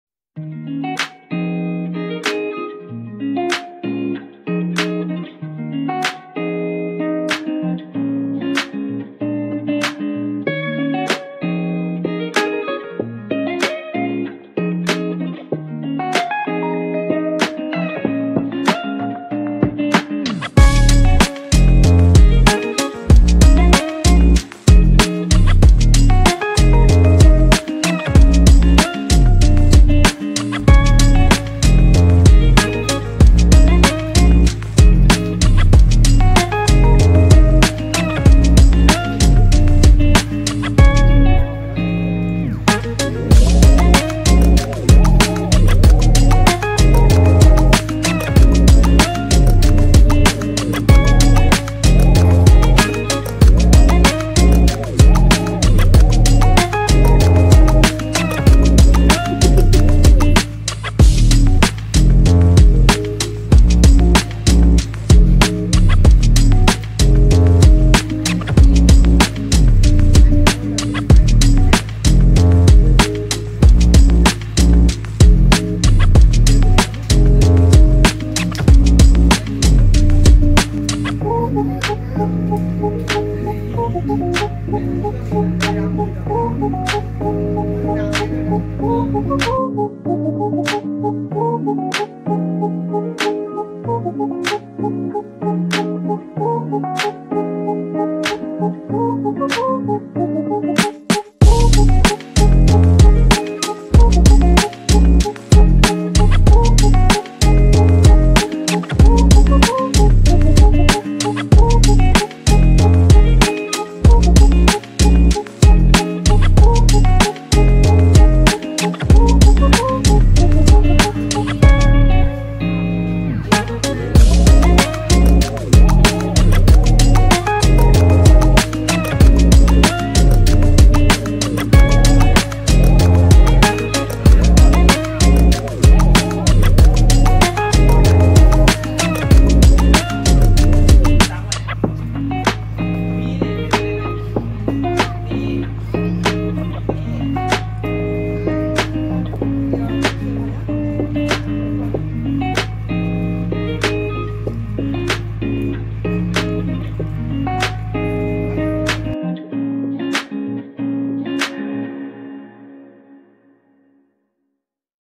Thailand, RUSA DEER, Endangered, Oasis Sea World, Chanthaburi
The Thai Navy has many conservation/preservation projects within the Gulf of Thailand.  RUSA Deer are critically endangered mammals.  They are safe here at Oasis Sea World and are slowly repopulating in peace.